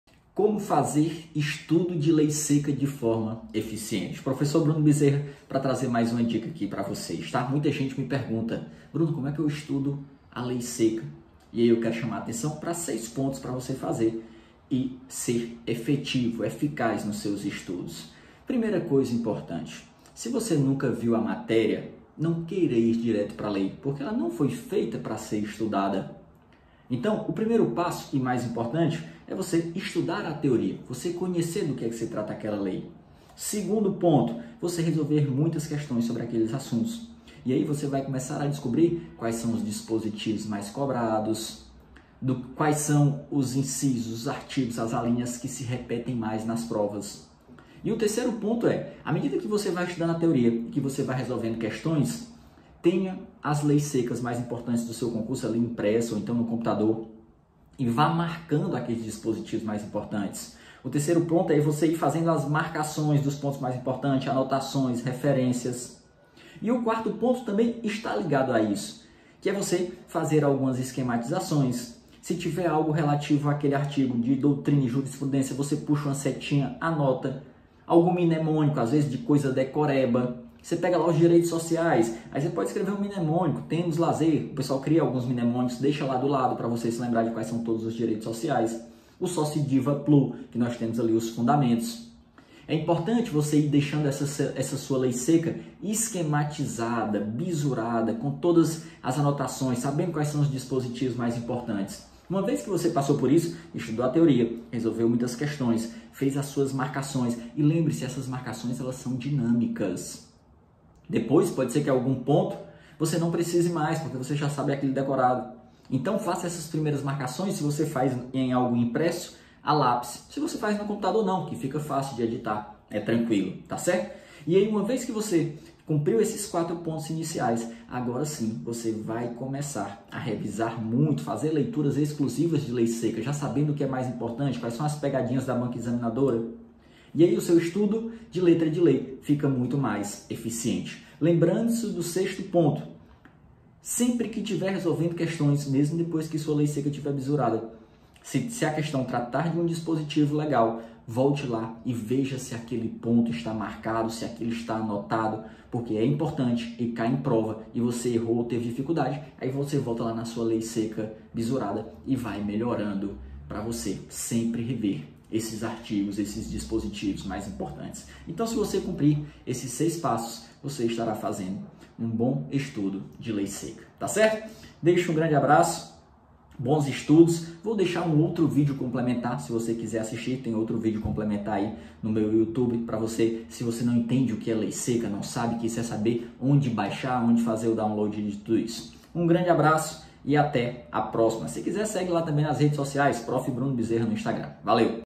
Como Estudar Lei Seca para Concursos Públicos | Guia Prático e Estratégico
Descubra as melhores estratégias para estudar a "lei seca" para concursos públicos neste guia prático.

Aprenda a maximizar seus estudos e compreender profundamente a legislação, essencial para sua aprovação.

Quer Estudar comigo?